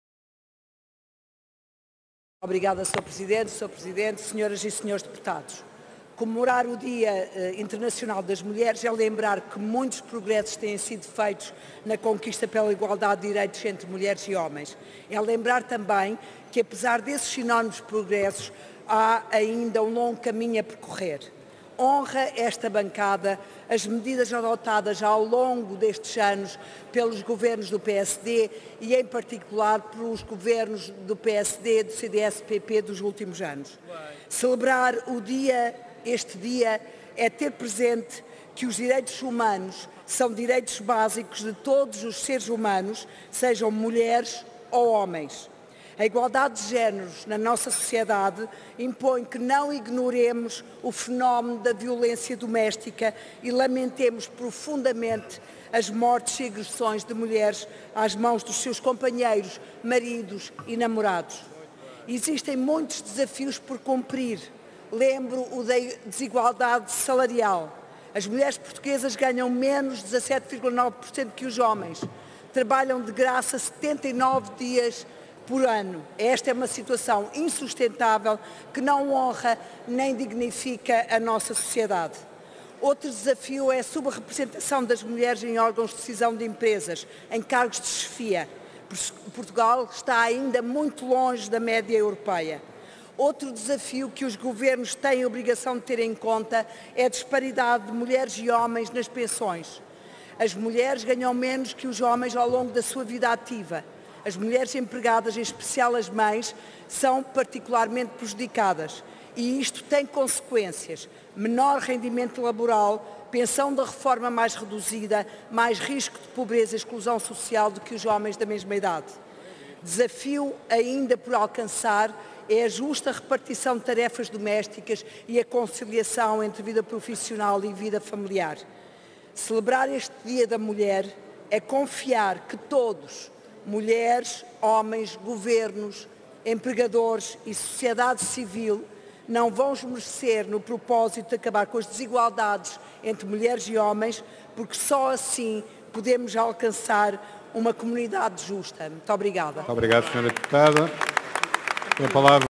Intervenção de Regina Bastos no Plenário
Intervenção no debate do Voto de Congratulação pela comemoração do Dia Internacional da Mulher.